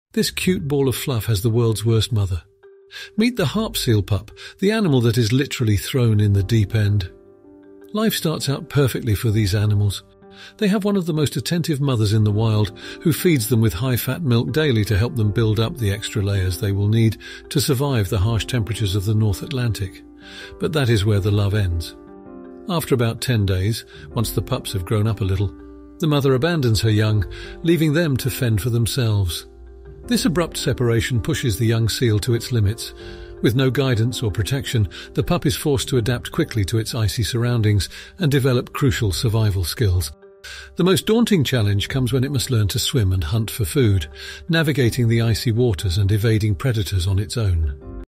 The World’s Worst Mother | Baby Harp Seal
This seal pup has a tough start to life. It's so sad.

Clips taken from:
Baby harp seal ch
National Geo
Verycuteseals
Animal Freedom
BBC Earth